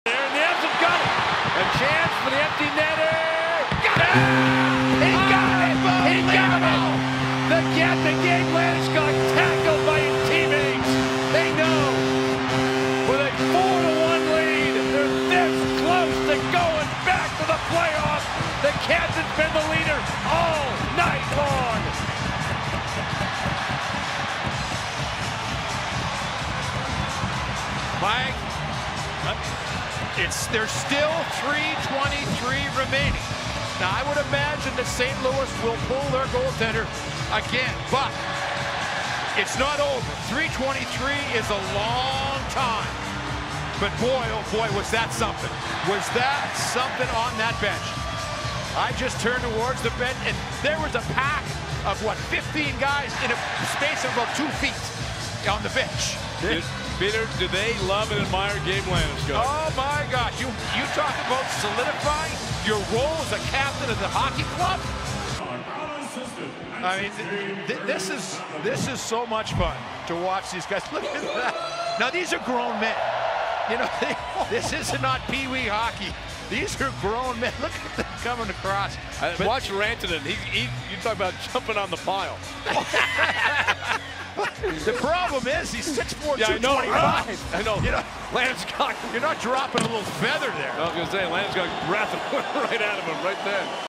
Avalanche dog pile on Gabriel Landeskog
The Colorado Avalanche pile on Gabriel Landeskog after he scores an empty net goal against the St. Louis Blues, as the team goes on to clinch a playoff spot.